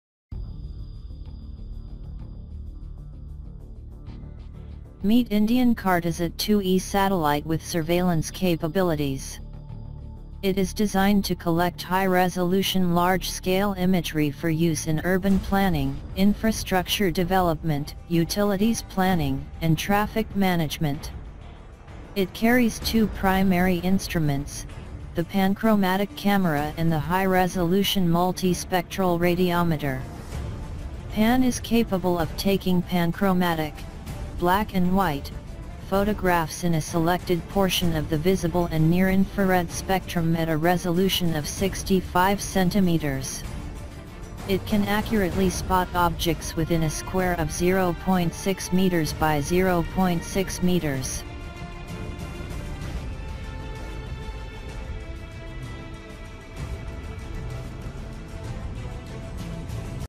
Meet Indian Cartosat-2E Satellites with Surveillance capabilities
It is designed to collect high-resolution, large-scale imagery for use in urban planning, infrastructure development, utilities planning, and traffic management.

It carries two primary instruments: the Panchromatic Camera (PAN) and the High-Resolution Multi-Spectral radiometer (HRMX). PAN is capable of taking panchromatic (black and white) photographs in a selected portion of the visible and near-infrared spectrum (0.50–0.85 µm) at a resolution of 65 centimetres (26 in). HRMX is a four-channel radiometer sensitive across the entire visible spectrum and part of the near-infrared spectrum (0.43–0.90 µm) at a resolution of 2 metres (6.6 ft).

Indian CARTOSAT-2E Eye-In-The-Sky Satellites. Features surveillance capabilities. It can accurately spot objects within a square of 0.6m by 0.6m.